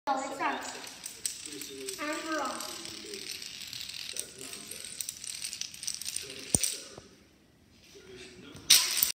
Dominos!
We bought dominos and made a lot of them this is 1 of them